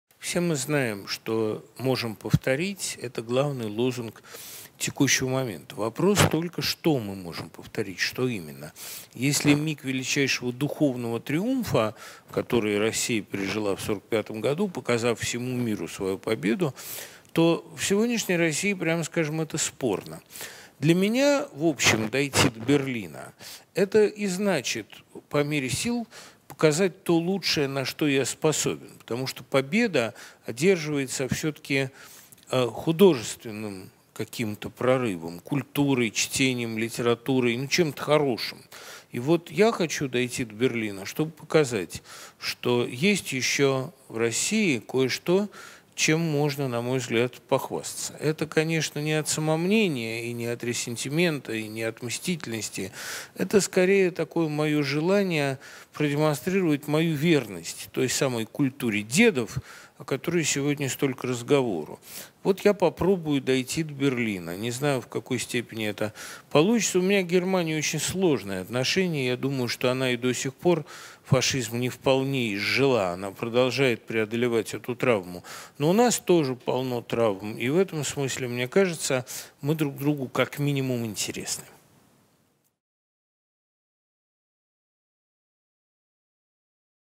Быков Берлин
22 апреля, воскресенье в 19.00 Дмитрий Быков «Дошёл до Берлина» Творческий вечер
22 апреля поэт и писатель Дмитрий Быков впервые выступит в Берлине с большим творческим вечером. Он прочтет свои стихи и прозу и ответит на вопросы слушателей.
Дмитрий Быков прежде всего - лирический поэт и поэтому приходите с ним познакомиться. Он будет читать свои старые, широко известные стихи и новую, только что написанную лирику. Смешную и грустную, плюс обязательный разговор с публикой, как всегда без посредников.

"Все мы знаем, что «можем повторить» – это главный лозунг текущего момента. Вопрос только: что мы можем повторить? Что именно? Если миг величайшего духовного триумфа, который Россия пережила в 45-м году, показав всему миру свою победу, то в сегодняшней России, прямо скажем, это спорно. Для меня, в общем, дойти до Берлина – это значит по мере сил показать то лучшее, на что я способен, потому что победа одерживается всё-таки каким- то художественным прорывом, культурой, чтением, литературой, ну, чем-то хорошим. И вот, я хочу дойти до Берлина, чтоб показать, что есть в России кое-что, чем можно, на мой взгляд, похвастаться. Это, конечно, не от самомнения и не от ресентимента, не от мстительности, это, скорее, такое моё желание продемонстрировать мою верность той самой "культуре дедов", о которой сегодня столько разговоров. Вот я попробую дойти до Берлина. Не знаю, в какой степени это получится. У меня к Германии очень сложное отношение, я думаю, что она и до сих пор фашизм не вполне изжила, она продолжает преодолевать эту травму, но у нас тоже полно травм, и в этом смысле, мне кажется, мы друг другу, как минимум, интересны." - Дм. Быков